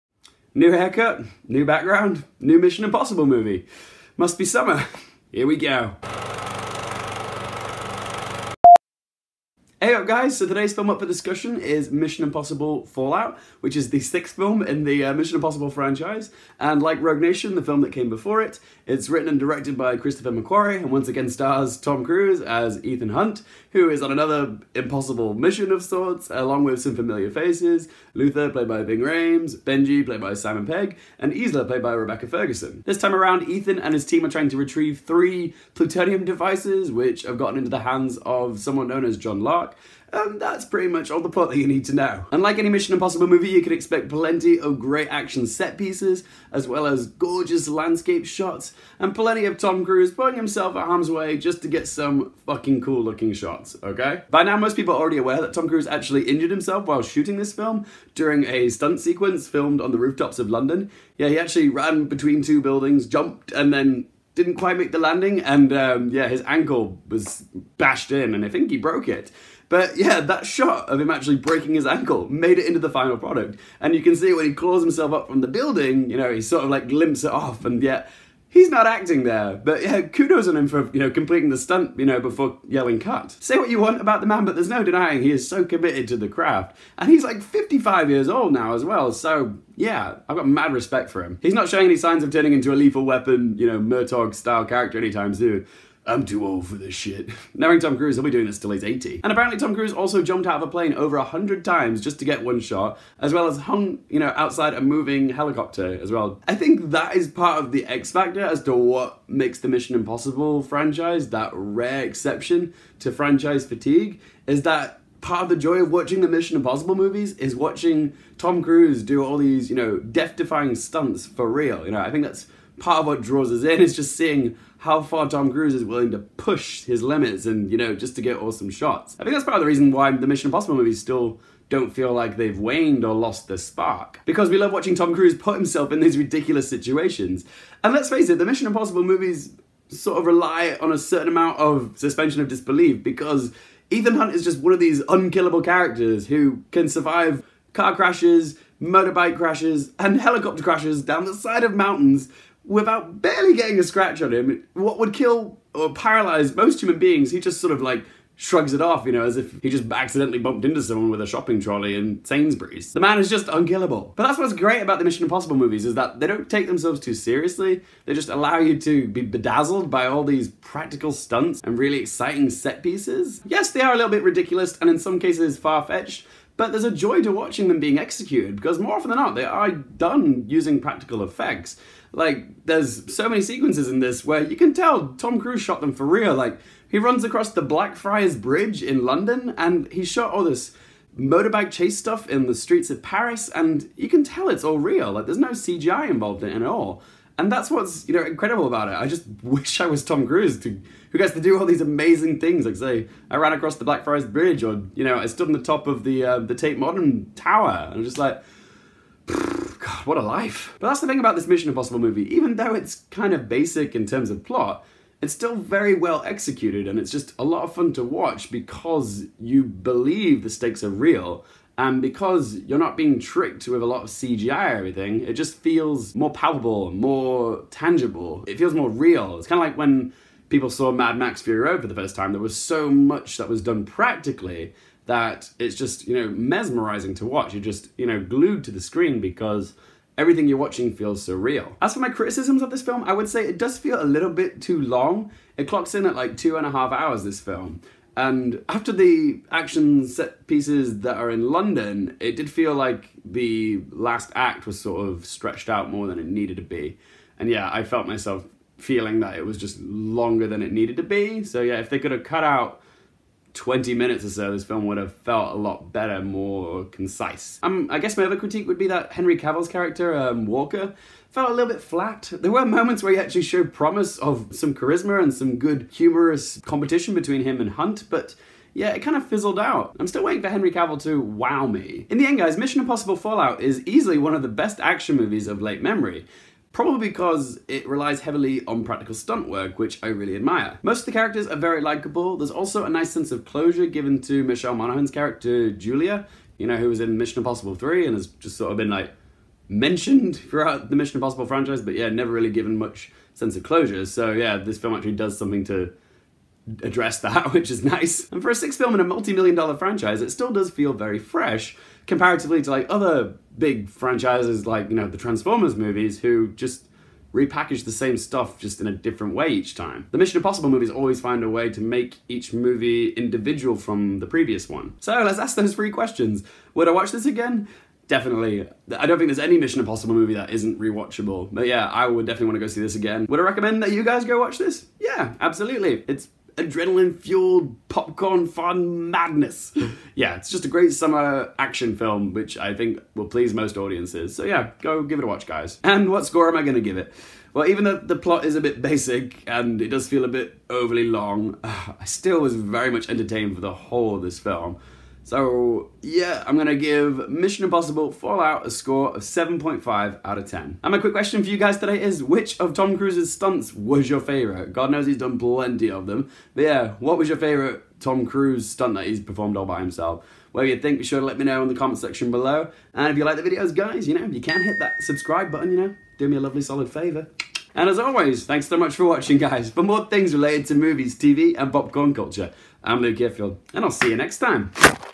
Mission Impossible: Fallout - Film Review
Tom Cruise is back as Ethan Hunt in the 6th film of the Mission Impossible franchise. Alongside returning characters Luther (Ving Rhames), Benjy (Simon Pegg) and Isla (Rebecca Ferguson) as well as newbie walker (Henry Cavill) watch the team try and stop a terrorist organisation from detonating three plutonium devices. Summer Action Movies don't get much better than this! Packed full of eye-dazzling stunts and crisp cinematography, this is another adrenaline-fuelled spy-caper that will be a big crowd-pleaser.